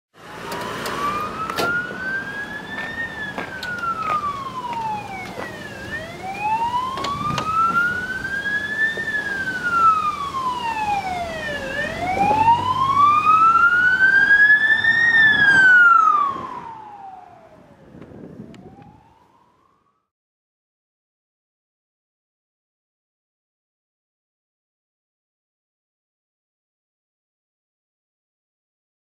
Landcruiser Ambulance Rushing Past [HD]
(16:9, 720P)
A four wheel drive Toyota Landcruiser ambulance speeding past with its lights and siren on it way to an emergency in Tully, Queensland.